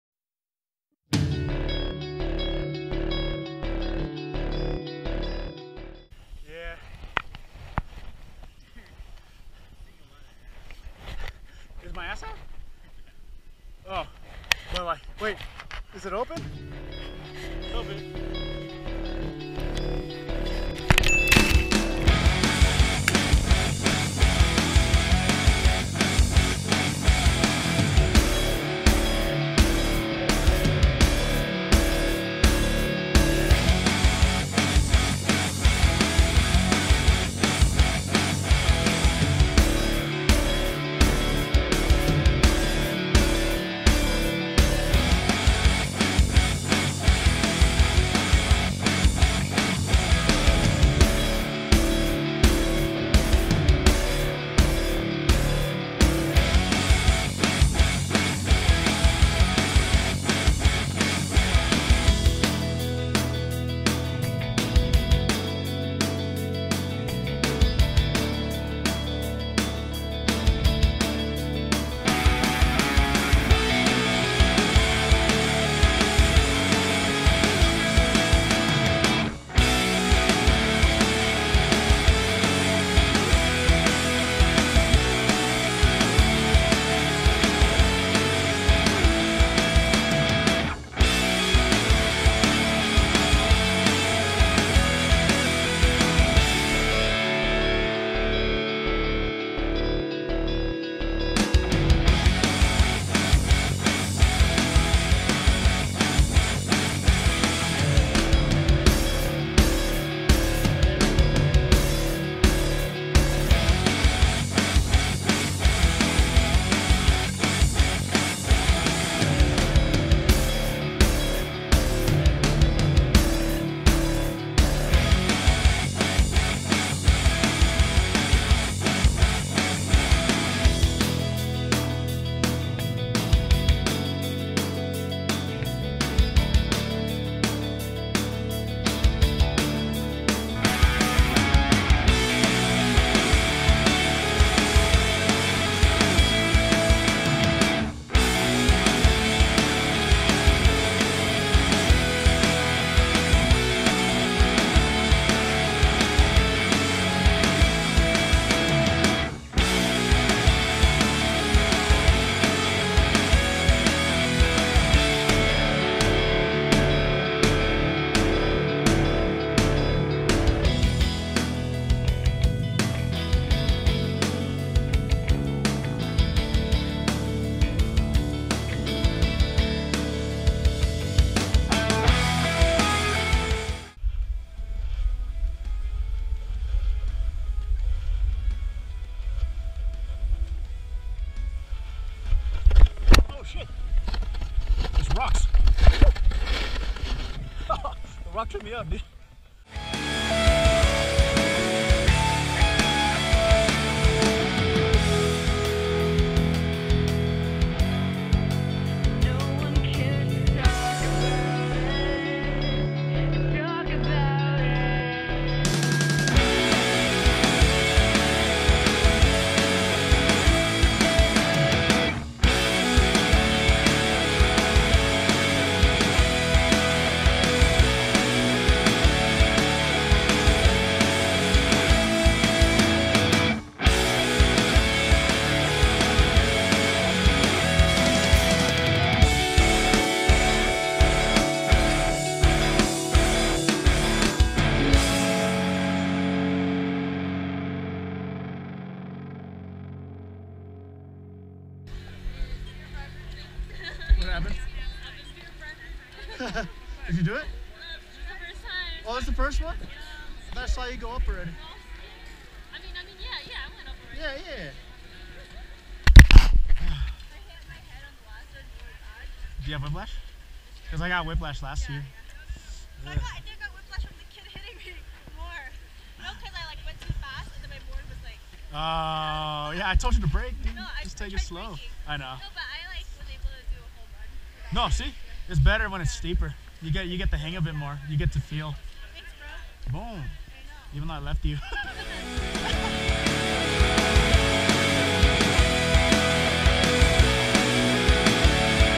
Mt. Charleston Part 1
Went snowboarding during winter break from pharmacy school! Good times and good vibes!